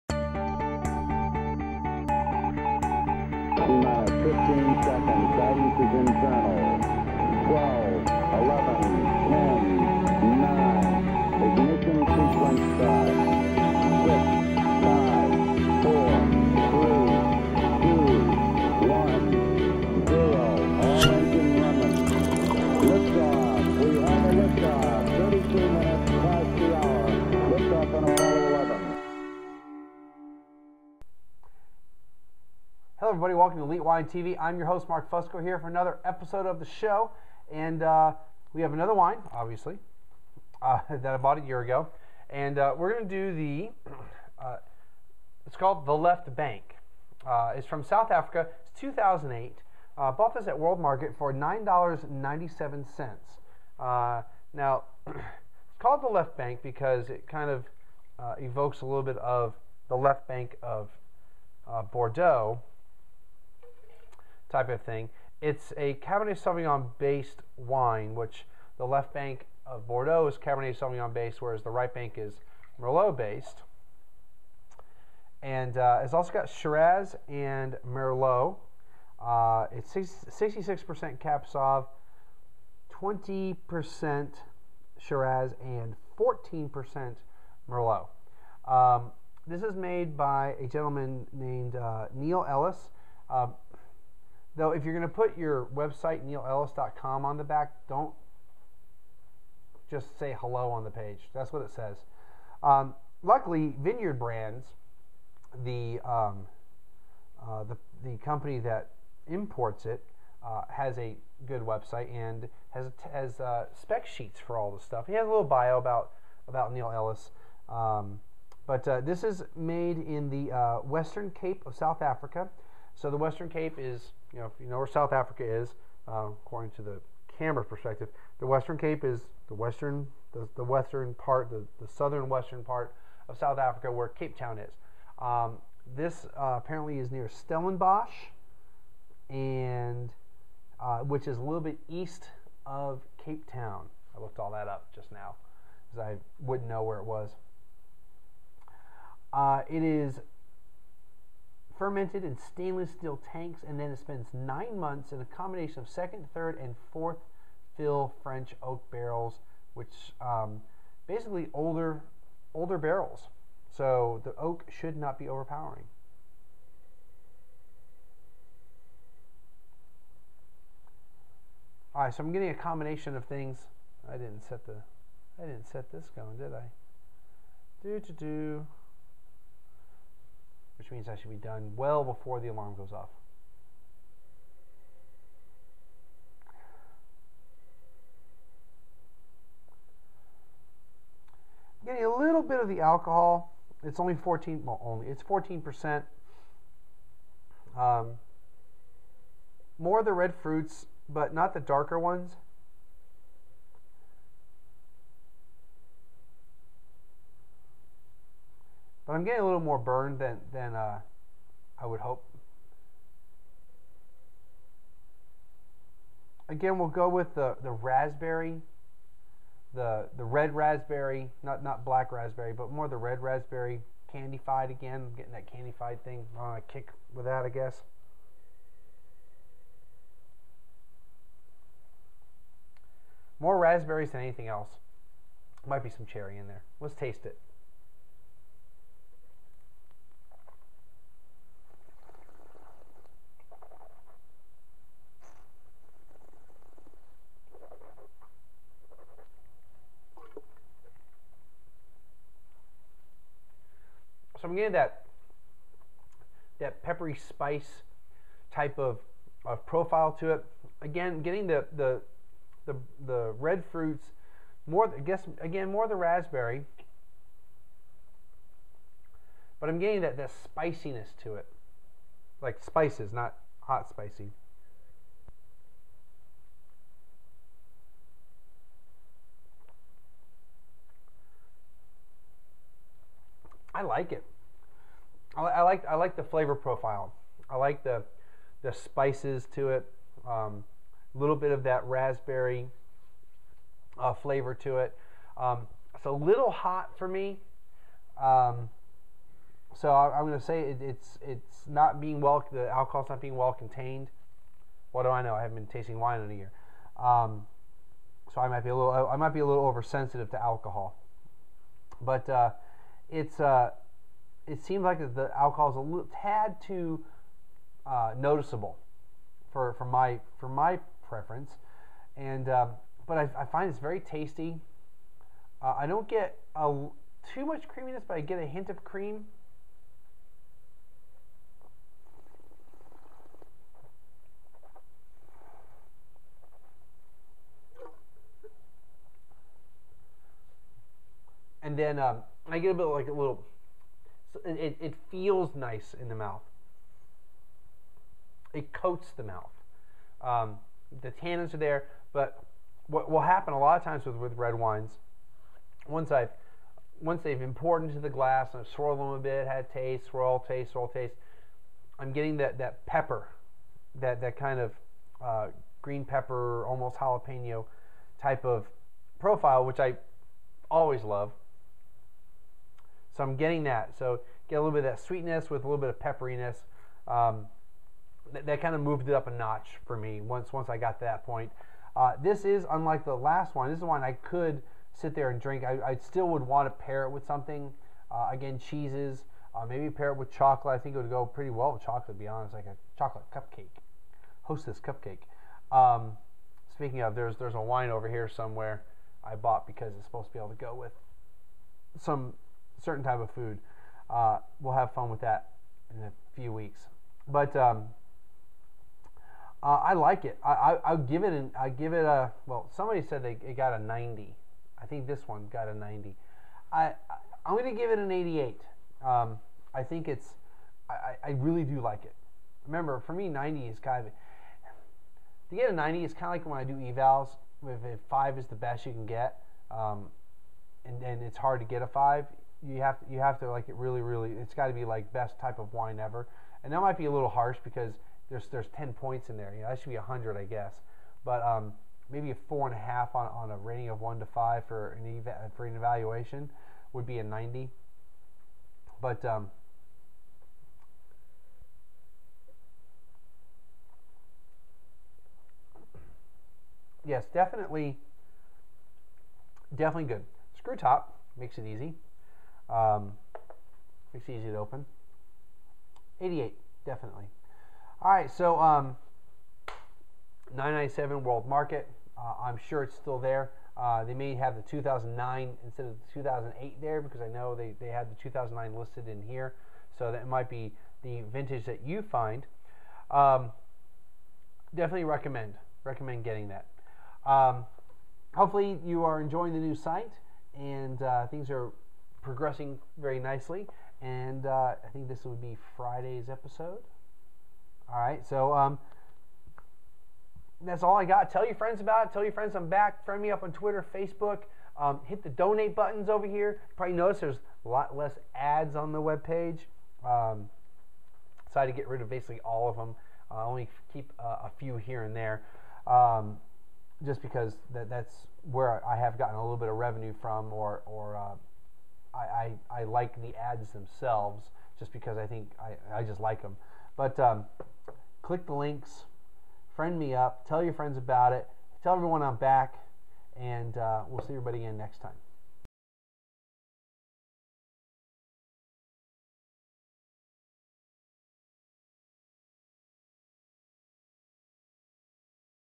The Left Bank - Episode #148
Author: Mark V. Fusco San Antonio, TX In the video wine review, Mark review the 2008 The Left Bank